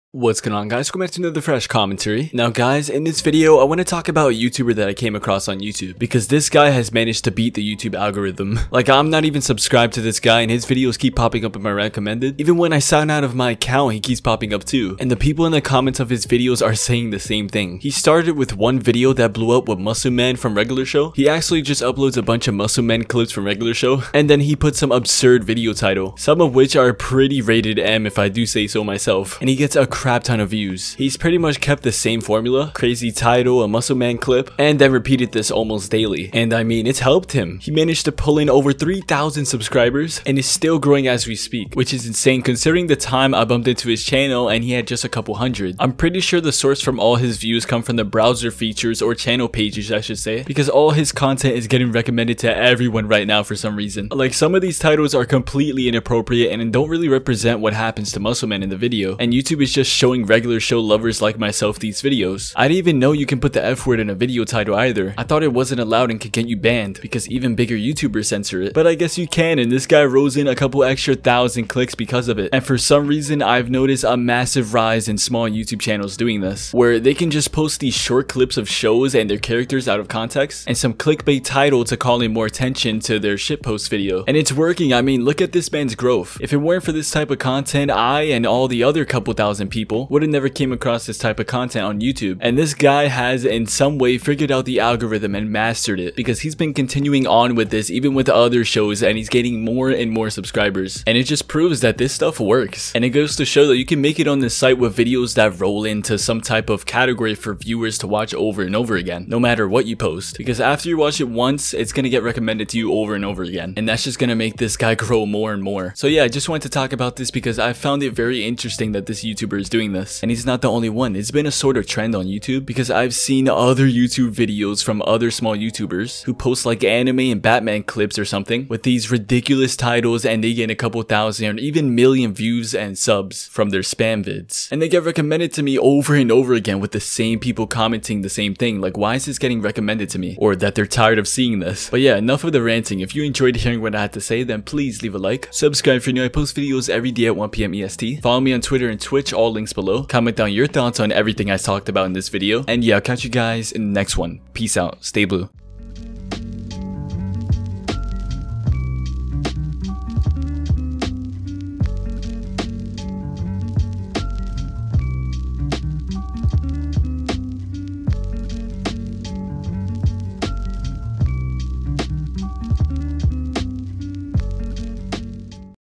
YouTuber Beats YouTube Algorithm With Muscle Man
In this video I talk about how a small youtuber has managed to beat the YouTube algorithm with muscle videos. It's a little odd to explain but I do so mentioning the YouTube recommended section. And how it's a bit funny because other small youtubers do the same thing. Just uploading short clips of characters from shows without context and getting a ton of views and growing from it.

Special Thanks to My Channel Members For Supporting Me 💙😊